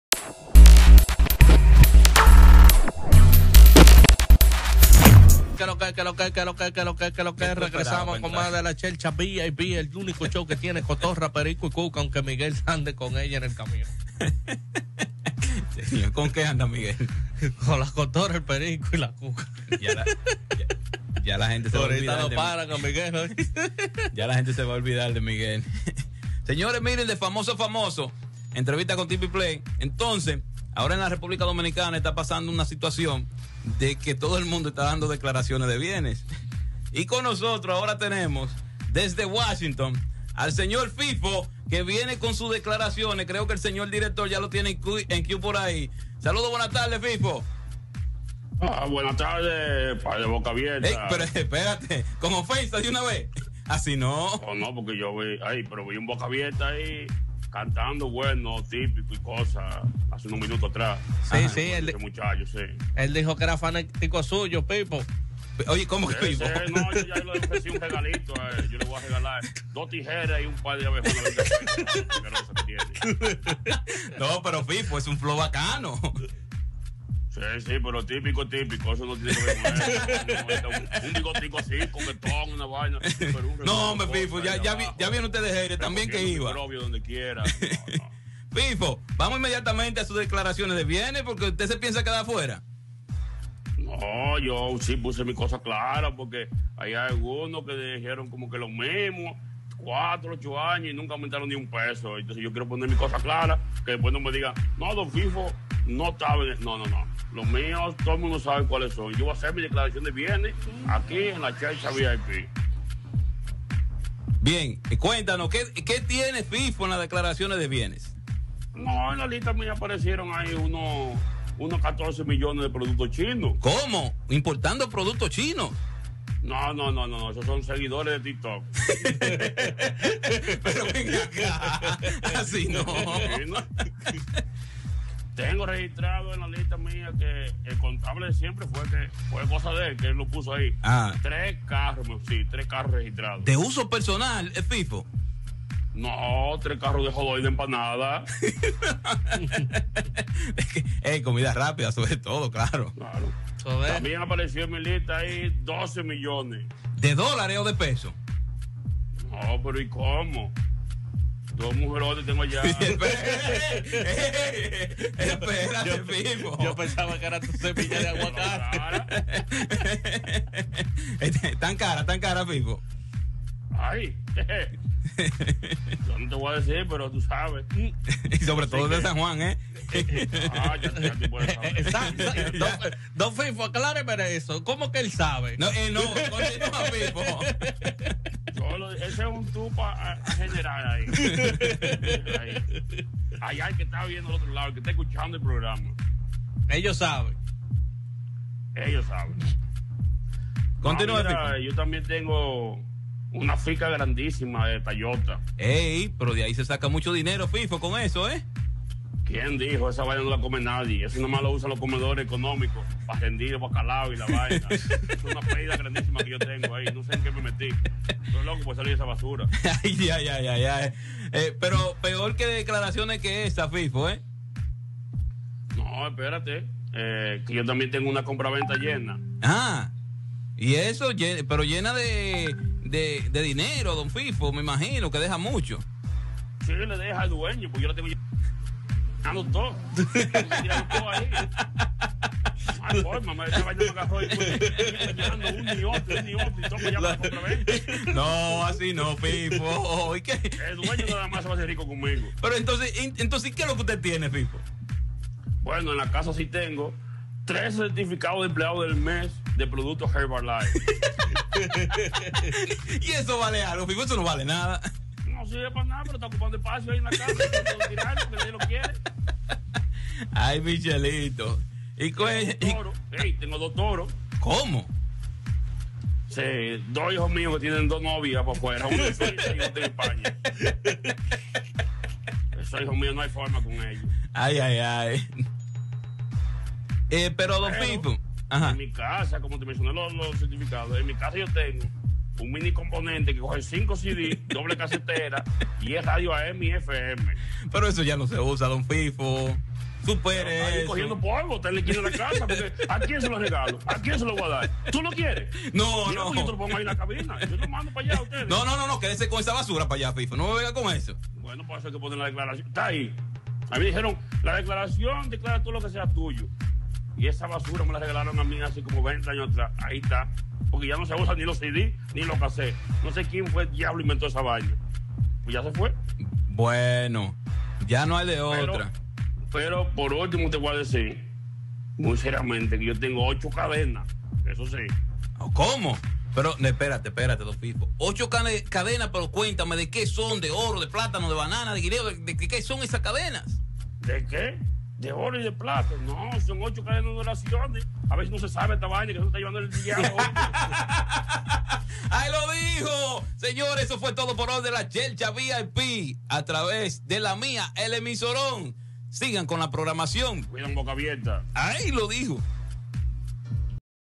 DECLARARACIONES DE BIENES DE DON FIFO
En la hercha vip nos presentaron la declaraciones de bienes del Don fifo del polaco show